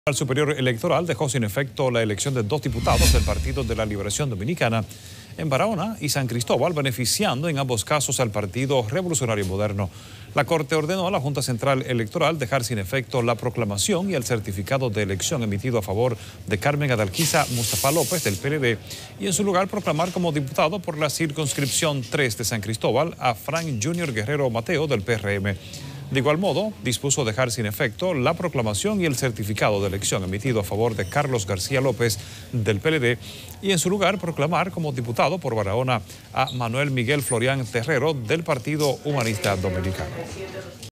TSE deja sin efecto elección dos diputaciones del PLD; las asigna al PRM
Santo Domingo, RD.- El Tribunal Superior Electoral (TSE) dejó sin efecto la elección de dos diputados del Partido de la Liberación Dominicana (PLD) en Barahona y San Cristóbal, beneficiando en ambos casos al Partido Revolucionario Moderno (PRM).

La corte ordenó a la Junta Central Electoral (JCE) dejar sin efecto la proclamación y el certificado de elección emitido a favor de Carmen Adalgisa Mustafá López del PLD, y en su lugar proclamar como diputado por la circunscripción 3, de San Cristóbal, a Frank Júnior Guerrero Mateo, del PRM.